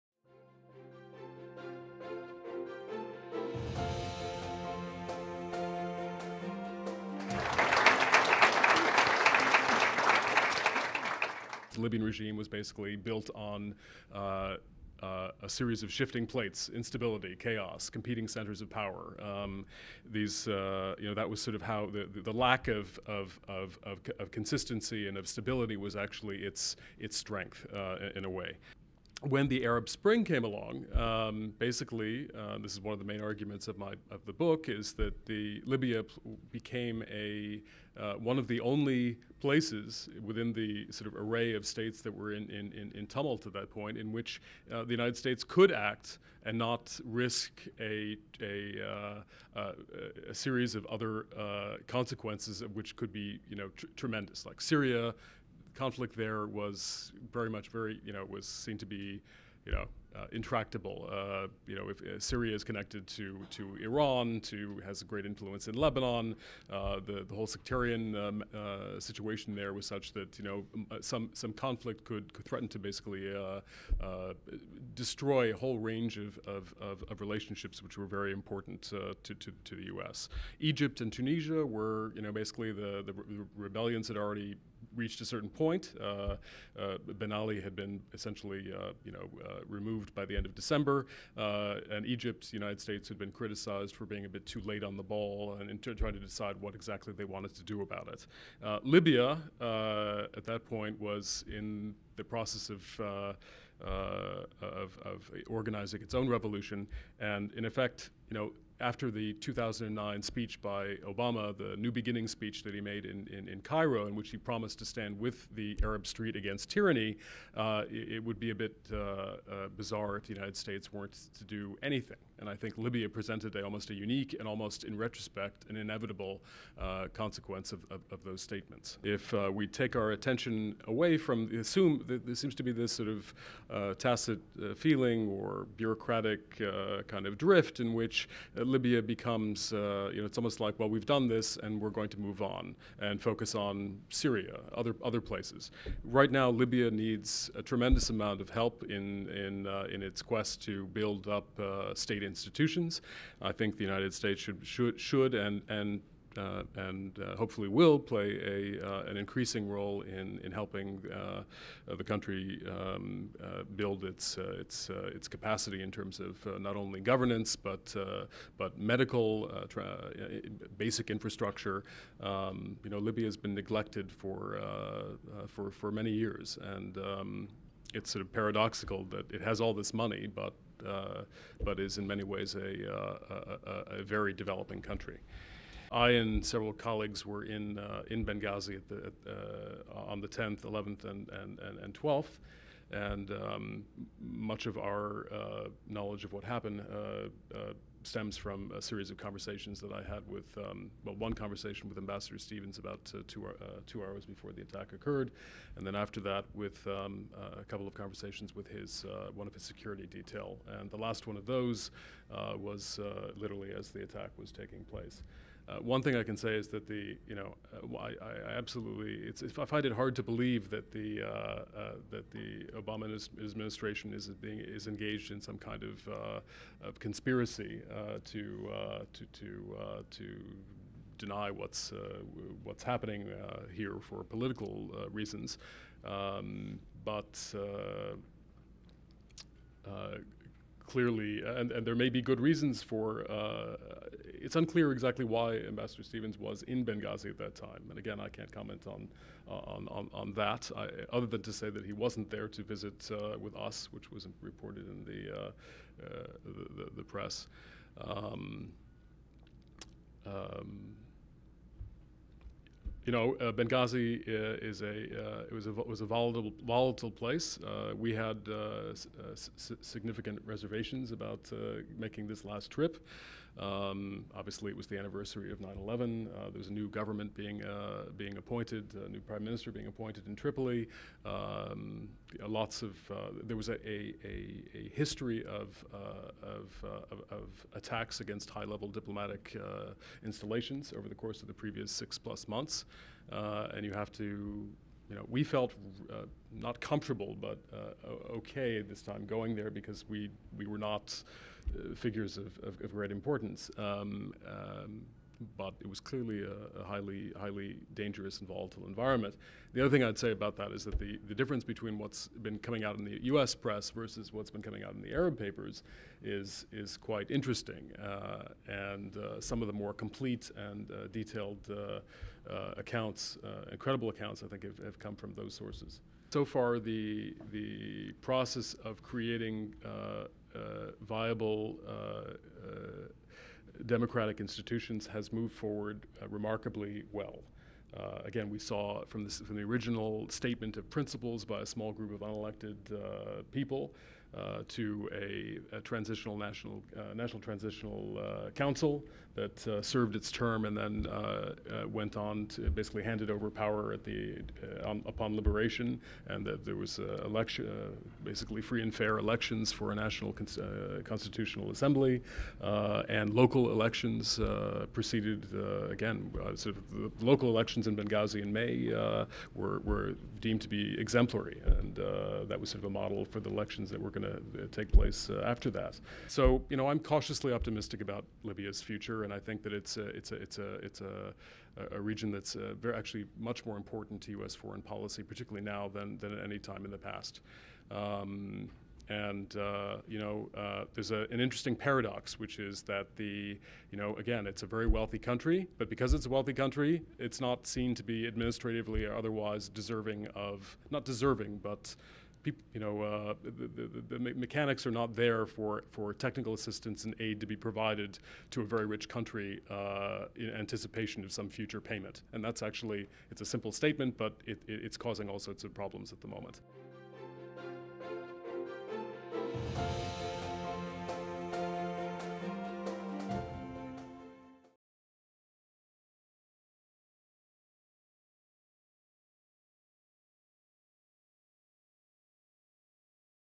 Ethan Chorin on Libya: Unraveling a Revolution In Brief
Speaker:
Ethan Chorin, Social Enterprise Fellow, Yale School of Management

The revolutions that have swept over the Middle East have captivated the world and changed the lives of millions in ways that have yet to be fully understood. What started in Tunisia and quickly spread across the Arab world, while relatively peaceful in some cases, quickly led to violent conflict in others—Libya in particular.

Drawing on extensive experience in the Middle East and Libya, and interviews with loyalists, rebels and senior US, EU and Libyan officials, Ethan Chorin will break down the Libyan revolution from the Gaddafi regime's "rehabilitation" and acceptance by the West to the regime's fall and possibilities for the future.